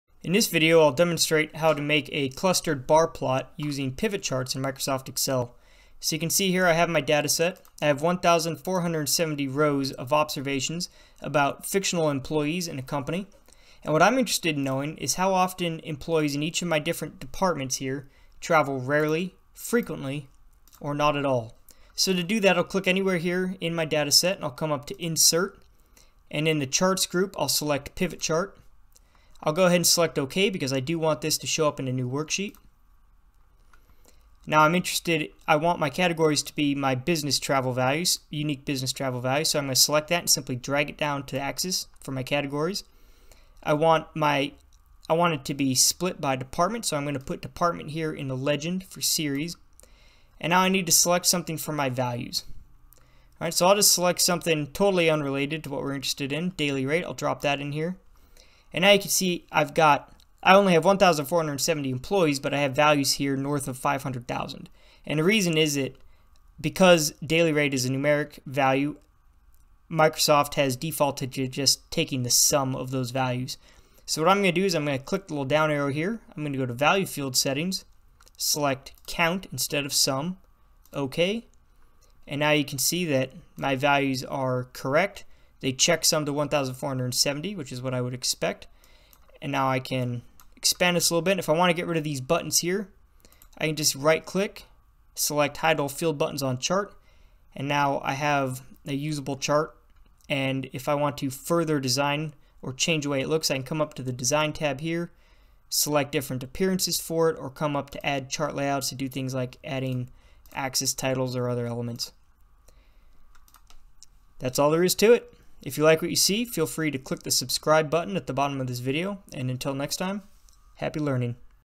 Making a Clustered Barplot (or Column Chart) in Microsoft Excel 365 using PivotChart
In this video I demonstrate how to create a clustered barplot (or column chart as its called in Excel) from a raw dataset using the PivotCharts functionality in Microsoft Excel 365.